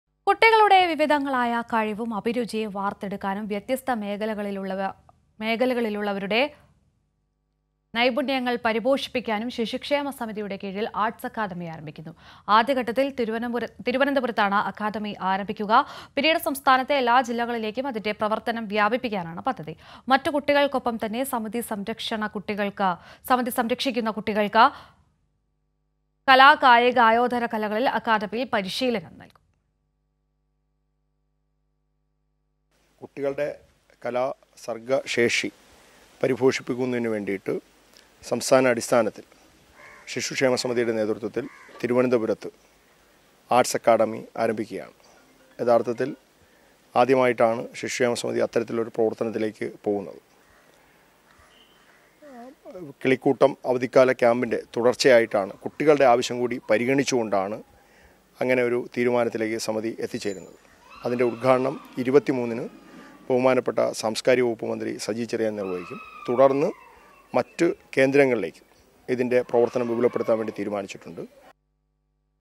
ശിശുക്ഷേമ സമിതിയുടെ കീഴിൽ ആർട്സ് അക്കാദമി ആരംഭിക്കുന്നു | Child welfare committee | Arts academy
കുട്ടികളുടെ വിവിധങ്ങളായ കഴിവും അഭിരുചിയും വാർത്തെടുക്കാനും വ്യത്യസ്ത മേഖലകളിലുള്ള അവരുടെ നൈപുണ്യങ്ങൾ പരിഭോഷിപ്പിക്കാനും ശിശുക്ഷേമ സമിതിയുടെ കീഴിൽ ആർട്സ് അക്കാദമി ആരംഭിക്കുന്നു
Arts academy getting started to help children to climb their dreams